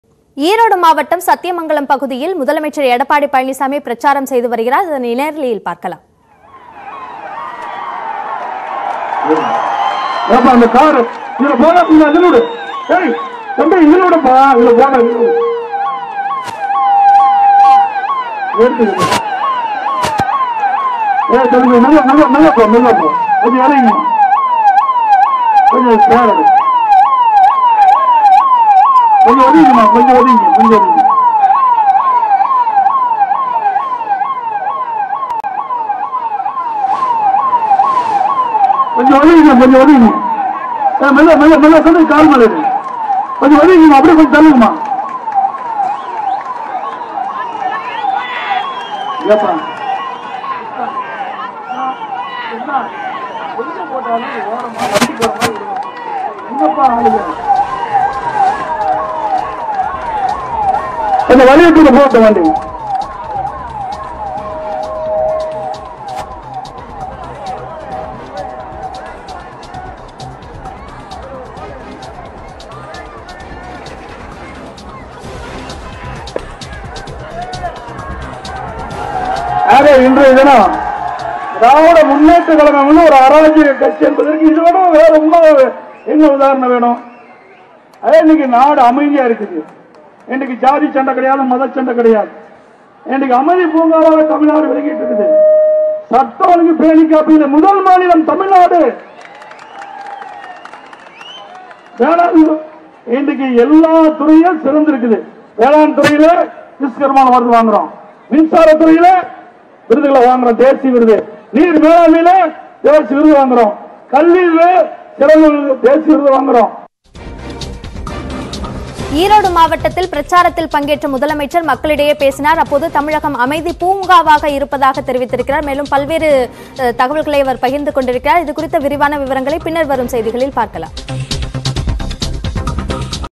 சத்தியமங்கலம் பகுதியில் முதலமைச்சர் எடப்பாடி பழனிசாமி பிரசாரம் | EPS | ADMK | TN Election 2021
ஈரோடு மாவட்டம் சத்தியமங்கலம் பகுதியில் முதலமைச்சர் எடப்பாடி பழனிசாமி பிரசாரம் செய்தார்.